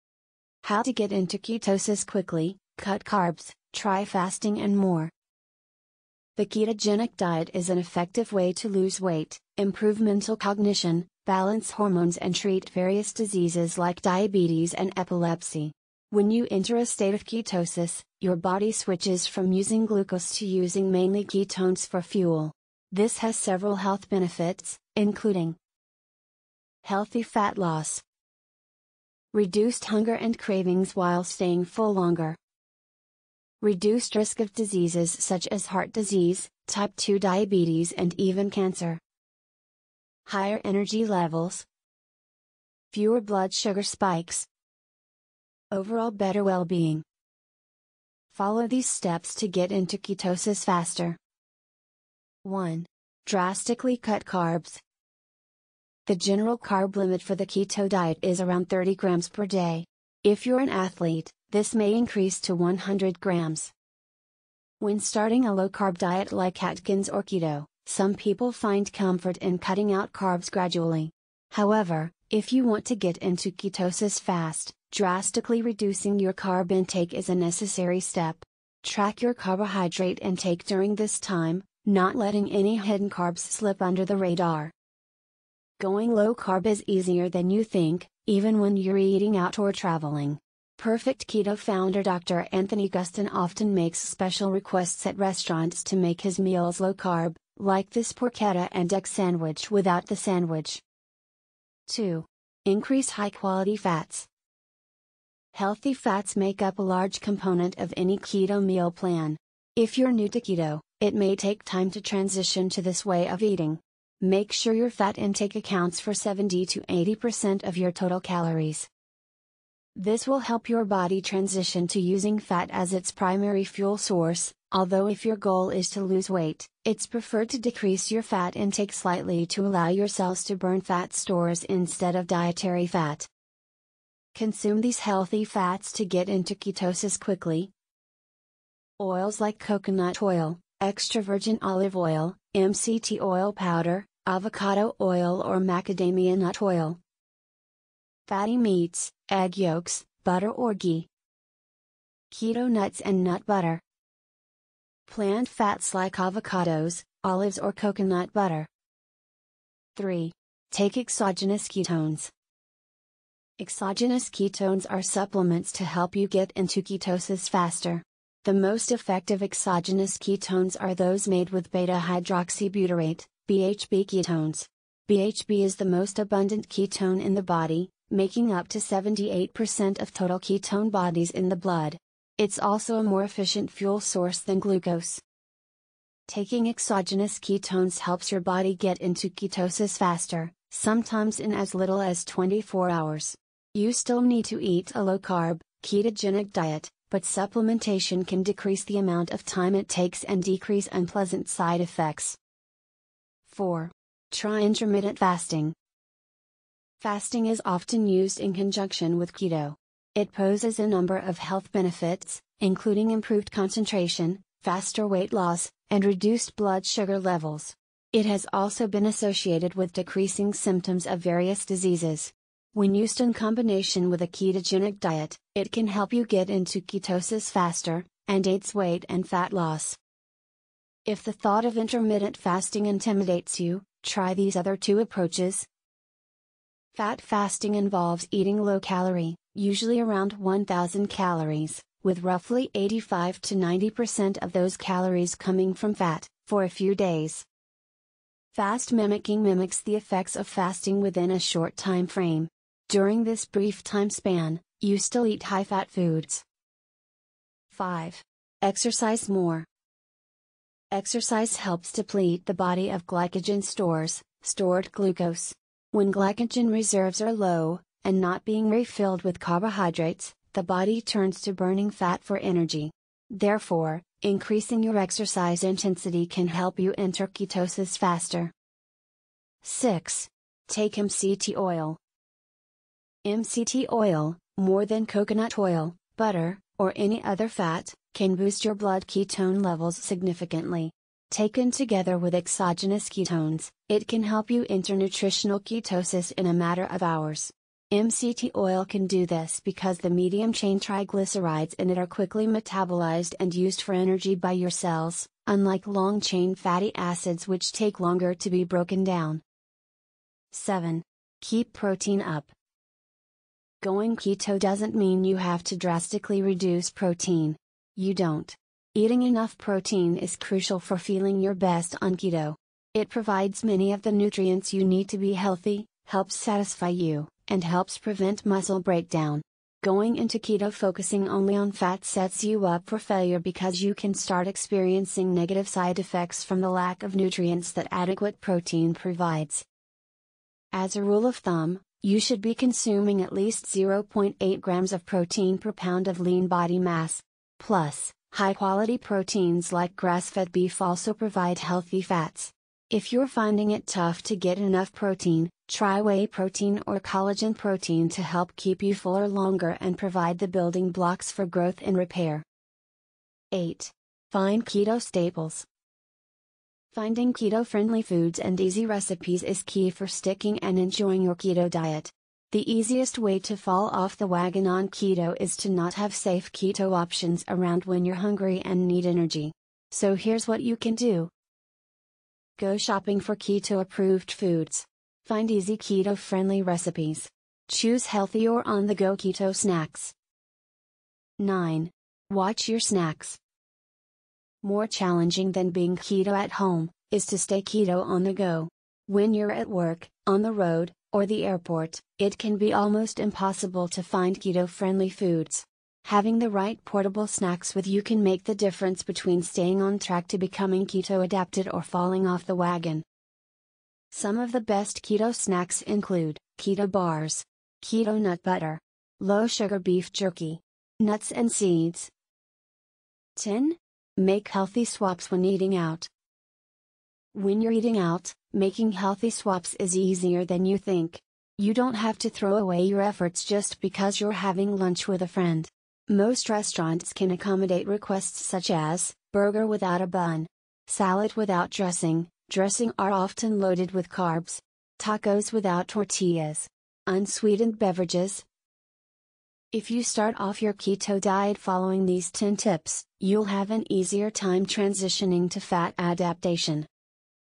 How To Get Into Ketosis Quickly: Cut Carbs, Try Fasting and More #28
The ketogenic diet is an effective way to lose weight, improve mental cognition, balance hormones and treat various diseases like diabetes and epilepsy.

1. Drastically Cut Carbs
2. Increase High-Quality Fats
3. Take Exogenous Ketones
4. Try Intermittent Fasting
5. Exercise More
6. Take MCT Oil
7. Keep Protein Up
8. Find Keto Staples
9. Watch Your Snacks
10. Make Healthy Swaps When Eating Out